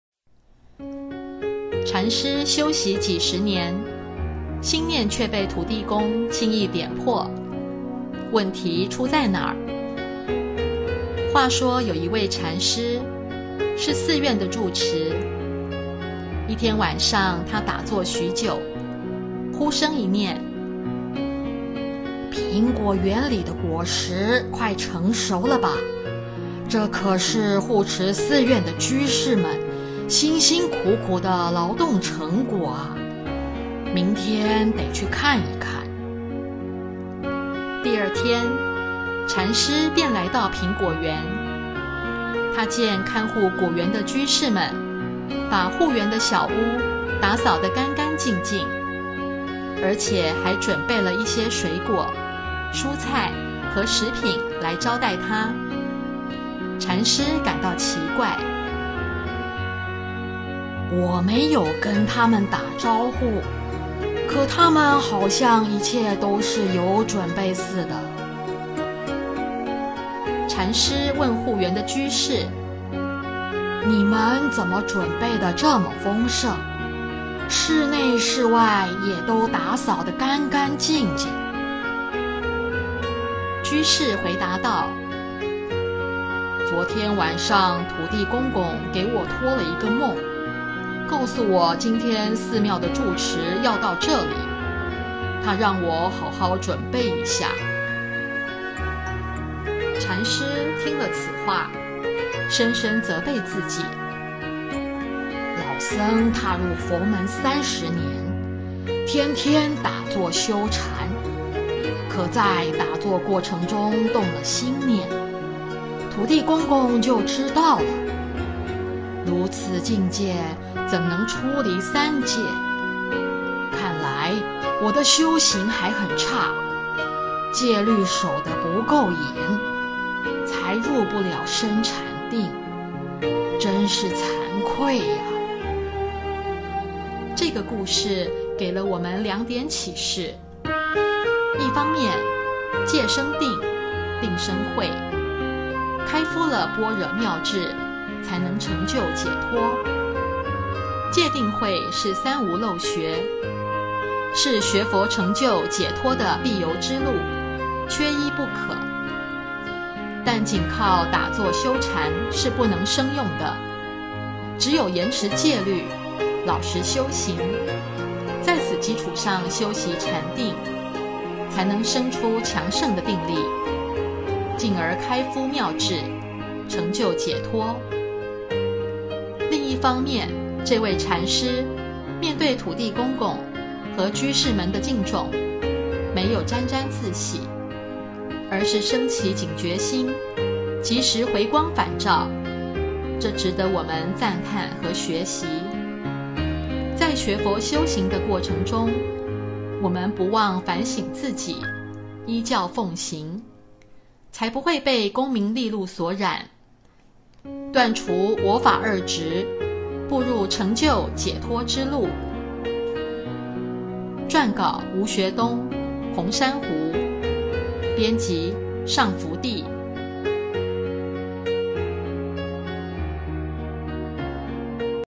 禪師修習幾十年，心念卻被土地公輕易點破，問題出在哪兒？
話說有一位禪師是寺院的主持。一天晚上他打坐許久，忽生一念：蘋果園裡的果實快成熟了吧，這可是護持寺院的居士們辛辛苦苦的勞動成果啊，明天得去看一看。

第二天，禪師便來到蘋果園。他見看護果園的居士們把護園的小屋打掃得乾乾淨淨，而且還準備了一些水果、蔬菜和食品來招待他。禪師感到奇怪：我沒有跟他們打招呼，可他們好像一切都是有準備似的？

禪師問護園的居士：“你們怎麼準備得這麼豐盛，室內室外也都打掃得乾乾淨淨？”

居士回答道：“昨天晚上土地公公給我托了一個夢，告訴我今天寺廟的住持要到這裡，他讓我好好準備一下。”

禪師聽了此話，深深責備自己：老僧踏入佛門三十年，天天打坐修禪，可在打坐過程中動了心念，土地公公就知道了。如此境界怎能出離三界，看來我的修行還很差，戒律守得不夠嚴，才入不了深禪定，真是慚愧啊！

這個故事給了我們兩點啟示：

一方面，戒生定，定生慧，開敷了般若妙智才能成就解脫。戒定慧是三無漏學，是學佛成就解脫的必由之路，缺一不可。但僅靠打坐禪修是不能生用的，只有嚴持戒律，老實修行，在此基礎上修習禪定，才能生出強盛的定力，進而開敷妙智，成就解脫。

另一方面，這位禪師面對土地公公和居士們的敬重，沒有沾沾自喜，而是生起警覺心，及時迴光返照，這值得我們讚歎和學習。在學佛修行的過程中，我們不忘反省自己，依教奉行，才不會被功名利祿所染，斷除我法二執，步入成就解脫之路。

撰稿：吳學東/紅珊瑚
編輯：上福地

本站註：佛弟子修學如來正法的知見與受用文章，其內容可能有若干錯誤，故只能作為參考交流、薰陶鼓勵之用，不為正見法理依據。